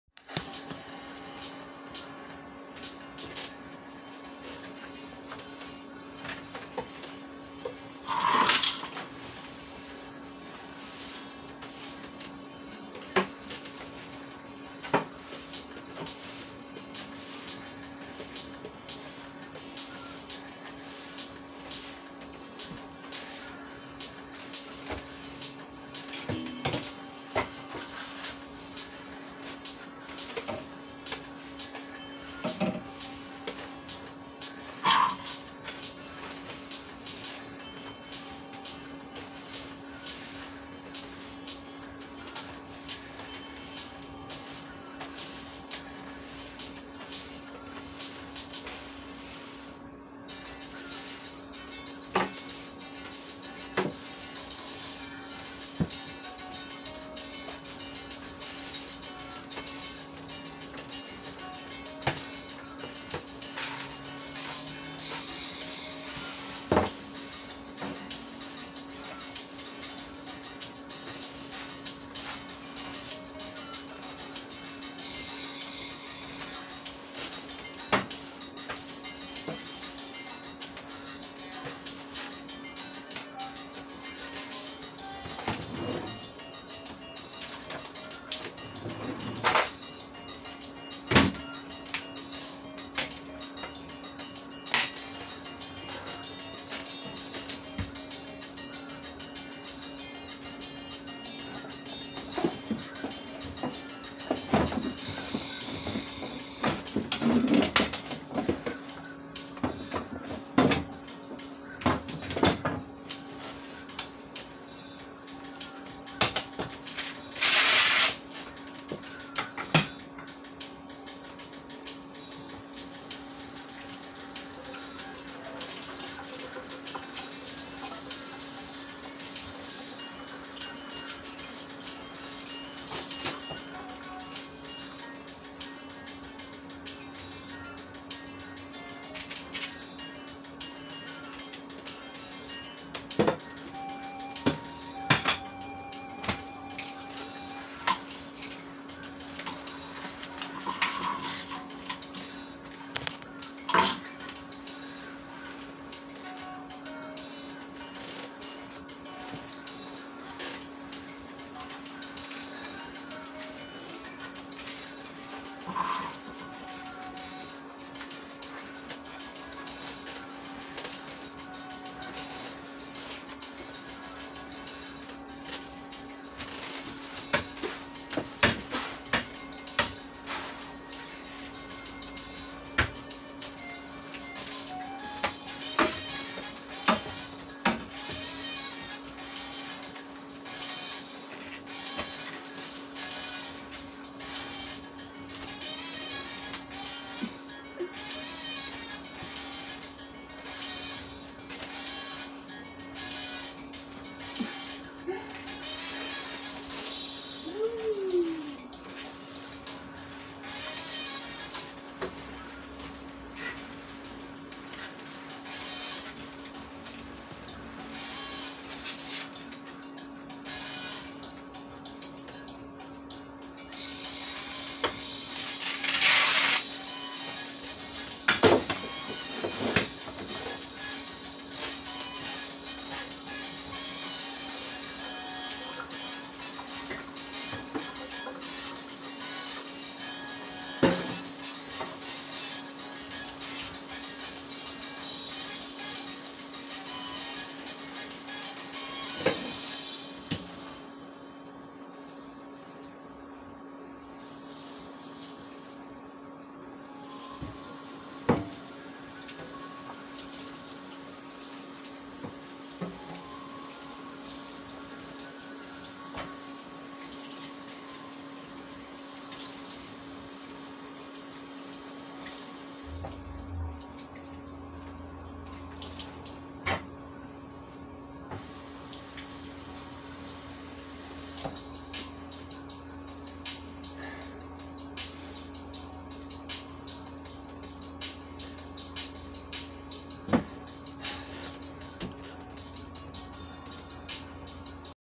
Clean With Me Part 2
Watch me finish cleaning for a job well done!

Wanna send me mail?
2615 Cortez Rd. W